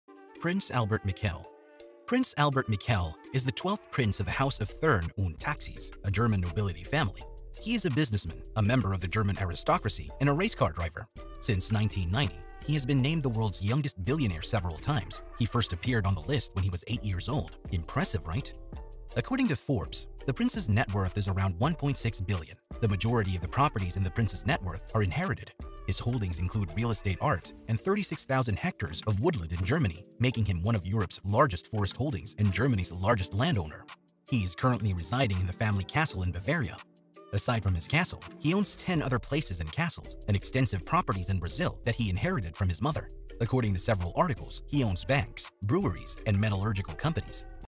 The BILLIONAIRE Lifestyle of Prince Albert Mikel
Are you curious about the lavish lifestyle of Prince Albert Mikel von Thurn und Taxis, the German Aristocrat? Watch this short video to find out more about his life of luxury and privilege!

In this video, we'll take a look at the life of Prince Albert Mikel, part of the Royal Family, and the House of Thurn and Taxis, and one of the richest princes and richest men in the world. After watching this short video, you'll know a bit more about his luxurious lifestyle! Enjoy!